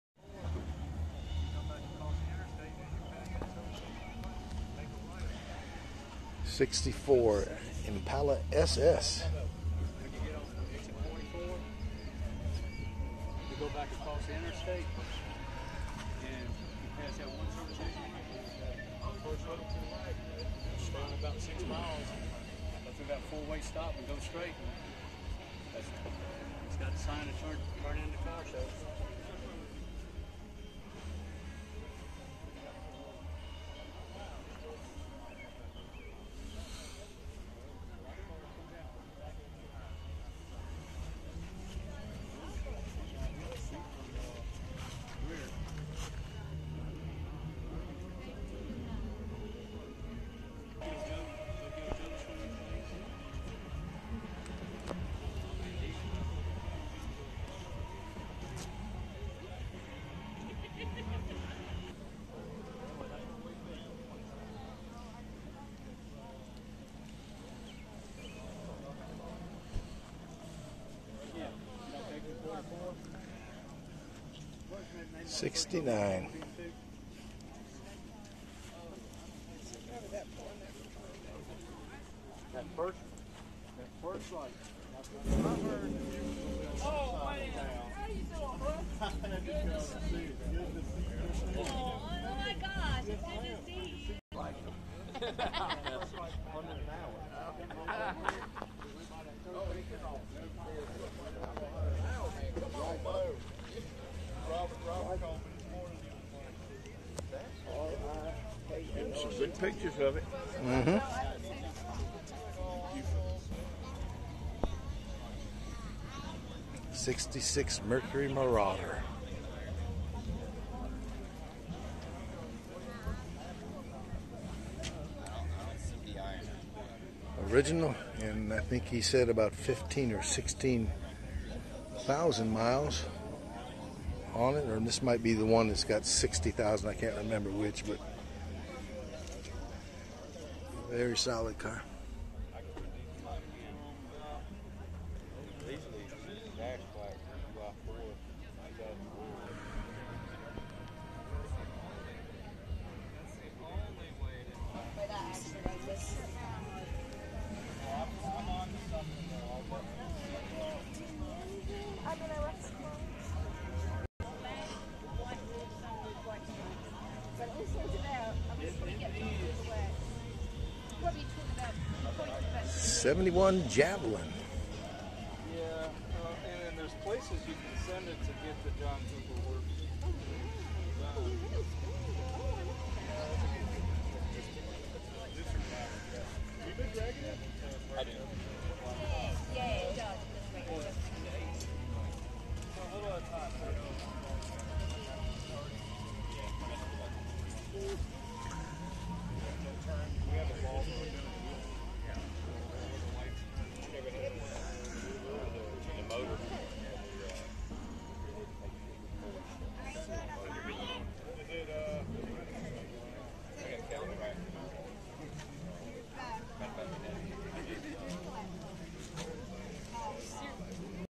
🔴 4th of July CAR SHOW 🔴 1964 Impala SS~ 1969 Camaro ~'66 Mercury ~ '71 AMC Javelin
Gilbert, SC PEACH FESTIVAL annual car show. 106 registered vehicles. In this video: '64 Impala SS 🔴 '69 Camaro convertible 🔴'66 Mercury Marauder 🔴 '71 AMC Javelin